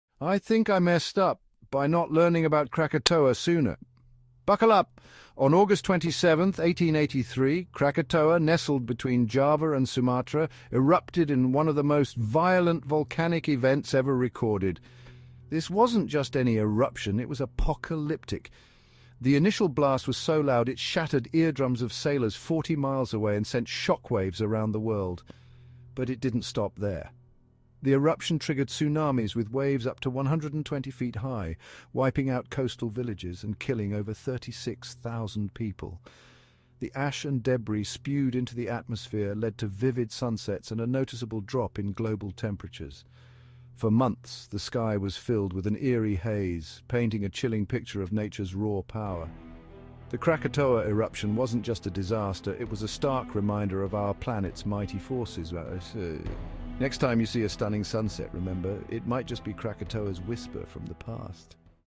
Today in History Episode: 9
On August 27, 1883, one of the most terrifying natural disasters in history occurred: the eruption of Krakatoa. This volcanic eruption, which took place in the Sunda Strait between the islands of Java and Sumatra in Indonesia, is considered one of the most violent volcanic events ever recorded.

The eruption produced a series of massive explosions that were heard thousands of miles away. The catastrophic event triggered tsunamis with waves up to 120 feet high, which caused widespread devastation and killed tens of thousands of people. The eruption and its aftermath led to significant climatic effects, including a drop in global temperatures and unusual weather patterns.

The sheer scale and impact of the Krakatoa eruption can be quite chilling, considering the destructive power of natural forces and the historical accounts of the devastation it caused.